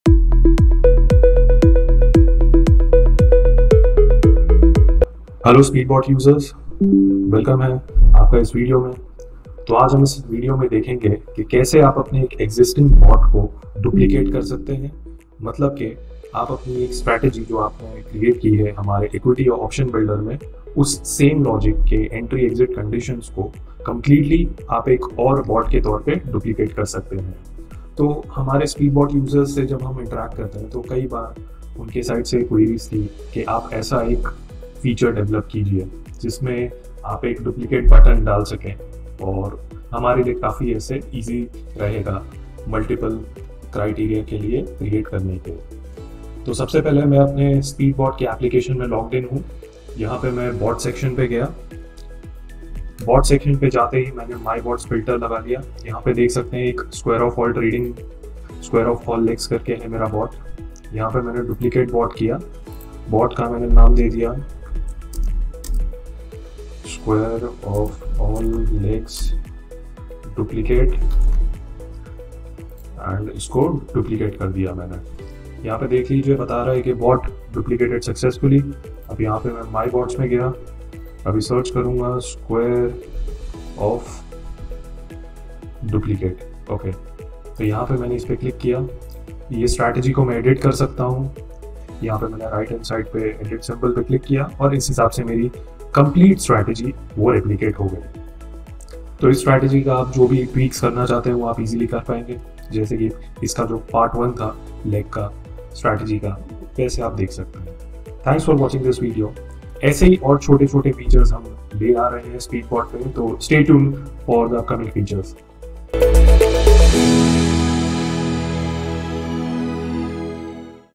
Duplicate Trading Bot Feature Detail Overview | SpeedBot
The duplicate bot feature in SpeedBot algo trading is a powerful functionality that allows users to effortlessly replicate successful trading strategies and actions. By selecting a bot to duplicate, users can automatically mirror the trading strategies in real-time, eliminating the need to create an entire strategy from scratch. This feature significantly speeds up the process of utilizing existing strategies with minimal modifications.

With the duplicate bot feature, users can leverage top-performing strategies and make customized adjustments to suit their needs. This capability has the potential to enhance their chances of success in the dynamic world of algorithmic trading. By leveraging proven strategies and making personalized tweaks, users can stay competitive and adapt to changing market conditions more effectively. The duplicate bot feature empowers users to optimize their trading approach and potentially achieve better results.
Get In Touch With Us!